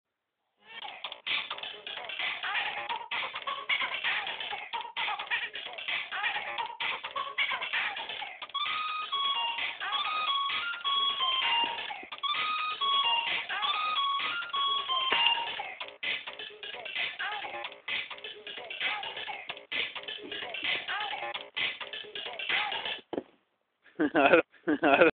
baile del chango tp .3gp
un dia sin nada qe hacer en la plazaa improbisaron un bailee el carlos (gamboa) y el cesar